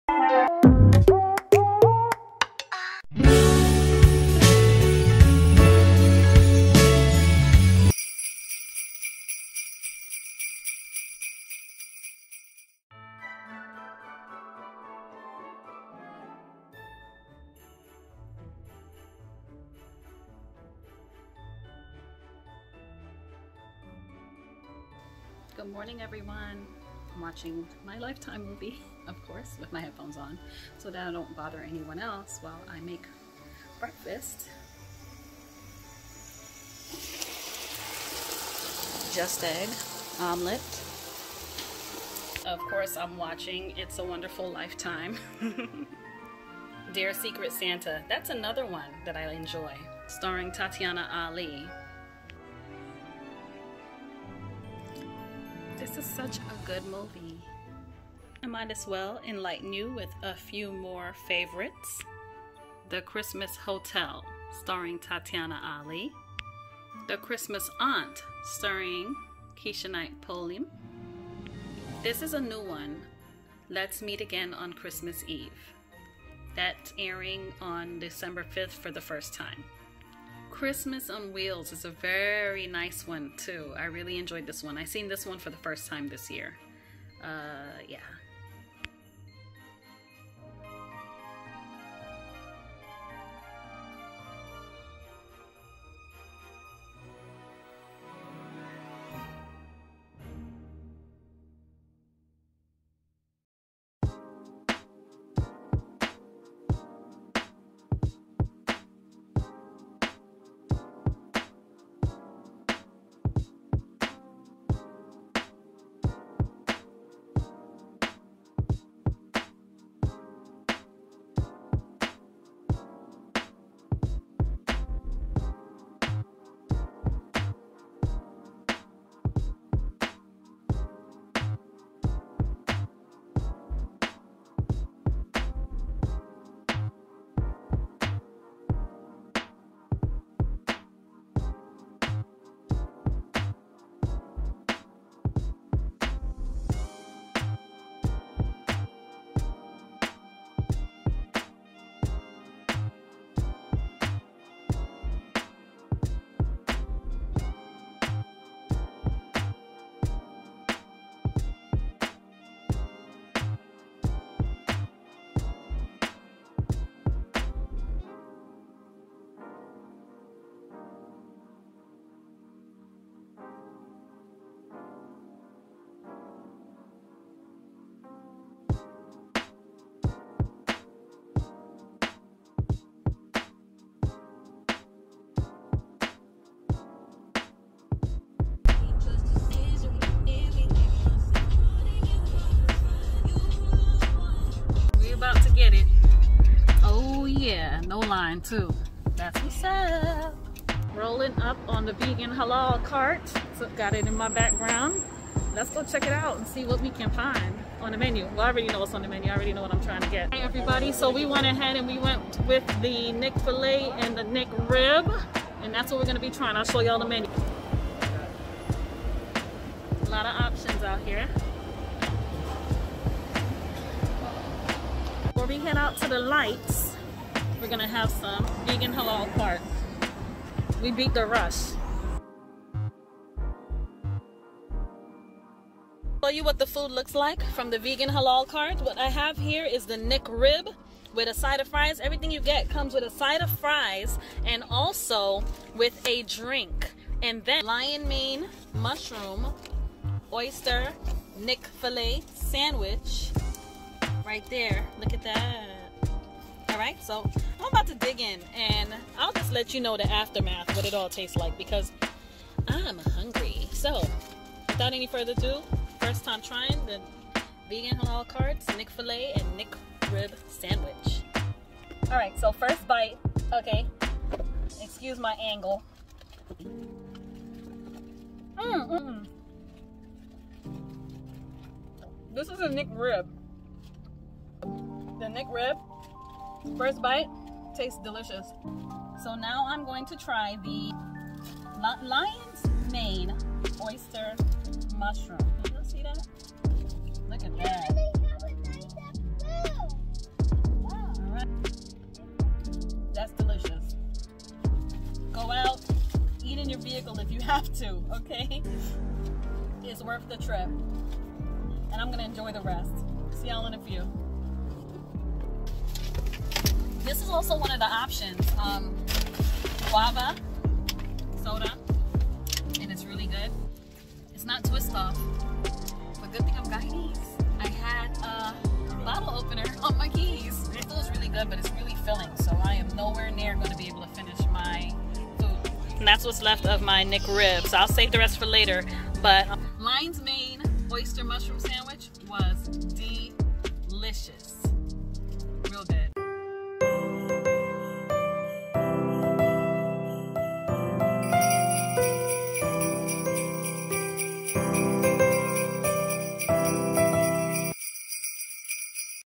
🎄Vlogmas Day 6 🎄- Spend The Day With Me
Lifetime Movie Reviews and a visit to The Vegan Halal Cart.

My MPow Bluetooth Headphones: